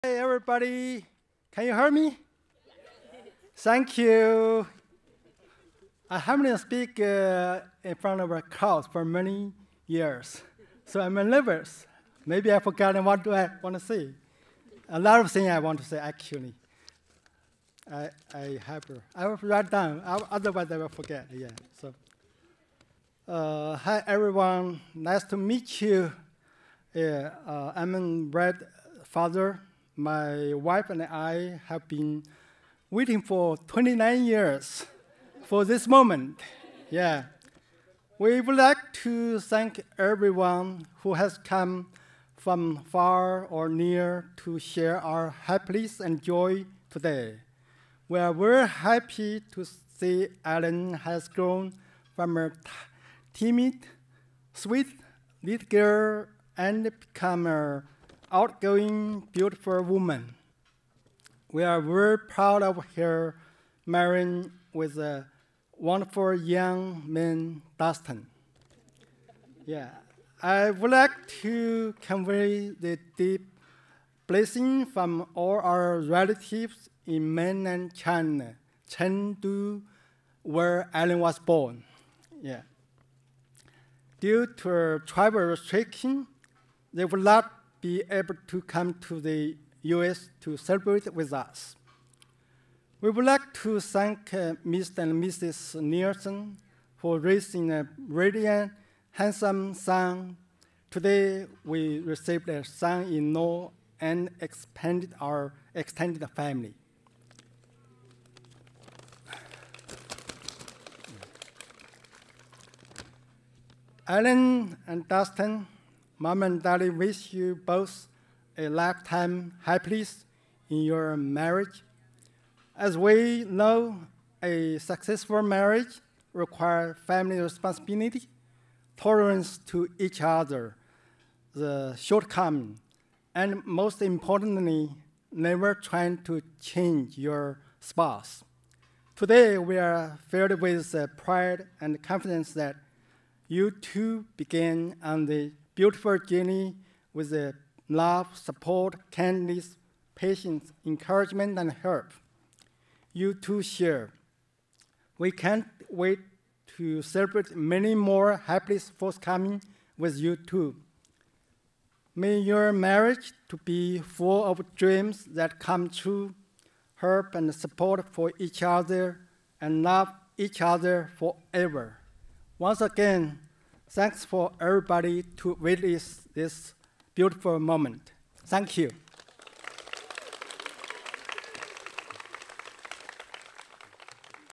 nervous immigrant dad gives sweetest wedding speech (father of the bride)
To my hero who moved across the world to give us more opportunities, always encourages me to dream big, and paved the path of entrepreneurship in our family.